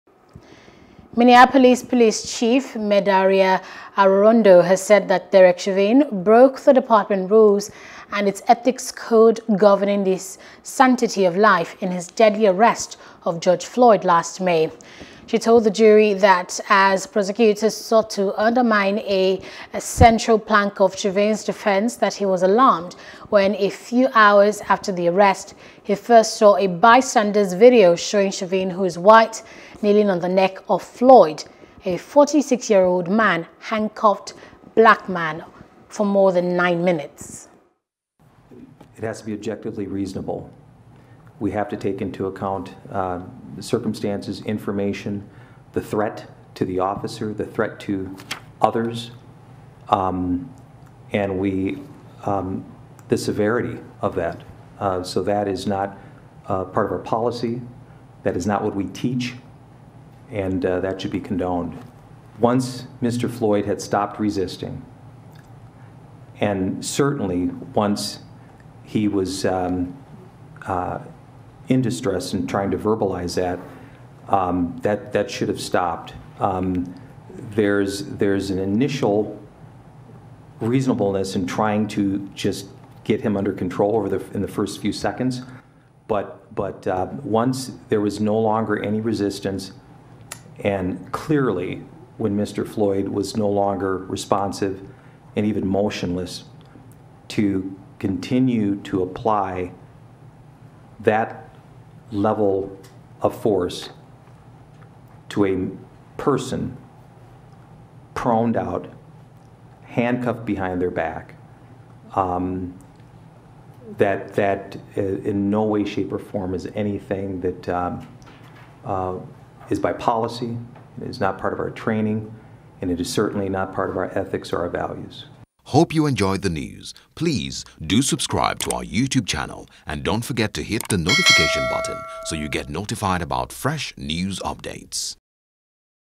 George Floyd: Minneapolis Police Chief Testifies Chauvin Violated Policy, Ethics Code | FOREIGN
Minneapolis Police Chief, Medaria Arradondo has stated that Derek Chauvin broke the department rules and its ethics code governing the "sanctity of life" in his deadly arrest of George Floyd last May.